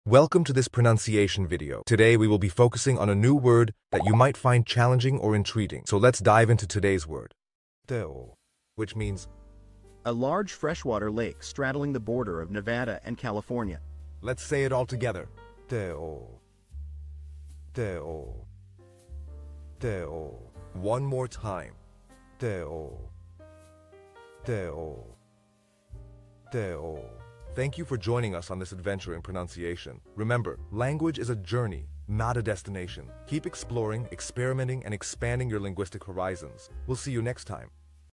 How to pronounce Tahoe
Master the Pronunciation of 'Tahoe WHICH MEANS TAHOE' - which means : The Large Freshwater Lake Straddling the Border of Nevada and California. 📝 with @PortuguesePronunciation 🎓🗣️ - [Your Guide to Portuguese]

Learn to pronounce 'Tahoe' perfectly in Portuguese with our easy and engaging tutorial by @PortuguesePronunciation! 📖📽️ In this comprehensive video, we provide you with native speaker examples 🗣️👂 and phonetic breakdowns 🎵 to ensure you get the pronunciation just right.

How to say TaHoe in Portuguese